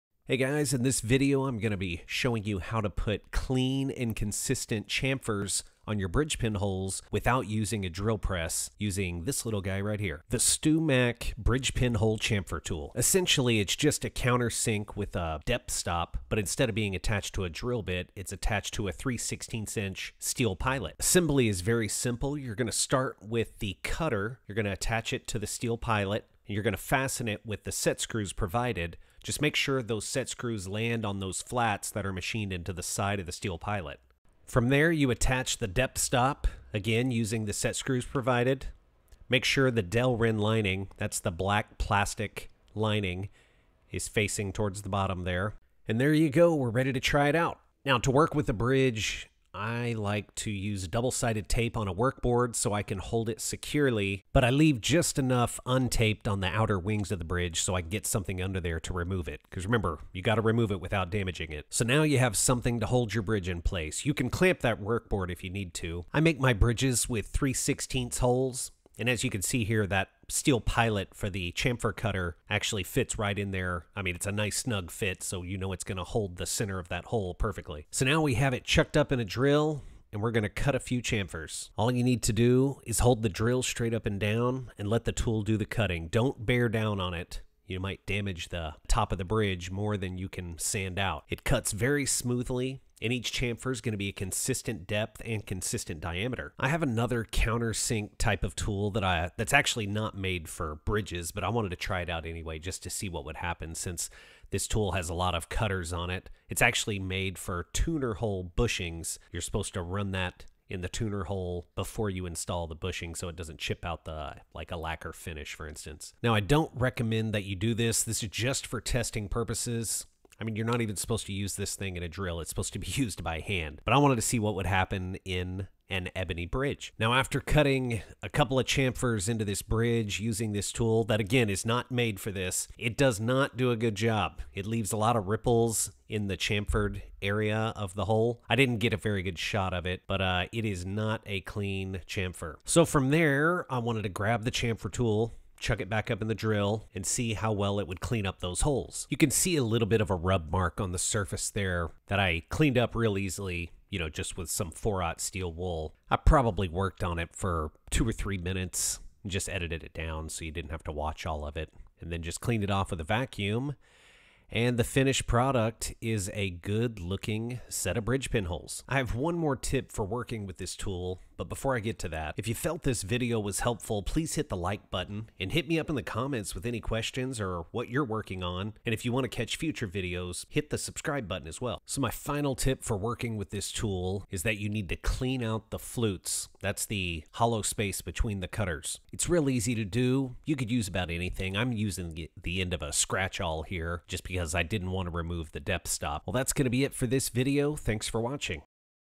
How To Chamfer Bridge Pin Holes (Without a Drill Press)
In this video Ill show you how to chamfer bridge pin holes using the Stew Mac Bridge Pin Hole Chamfer Tool. Its essentially a counter sink with a depth stop mounted on a 3/16 pilot. Perfect for guitar bridges! First you have to assemble it. Install the cutter, then the depth stop and you are ready to go. Works very well with a hand drill. Just hold the drill straight up and down and let the tool do the cutting. This tool makes it very easy to get clean cut chamfers on your bridge pin holes and they will be consistent in depth and diameter. No drill press needed.